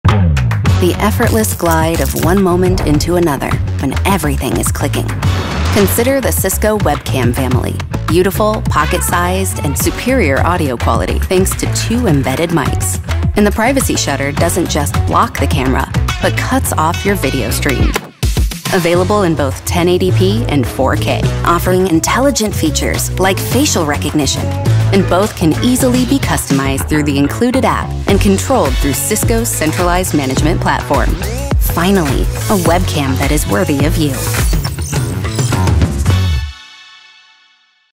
Cisco Desk Camera Series
Introducing the Cisco Desk Camera Series which includes two models, the Desk Camera 4K and 1080p. These USB webcams include features such as superior video quality, best-in-class privacy, easy setup, and the ability to customize image settings.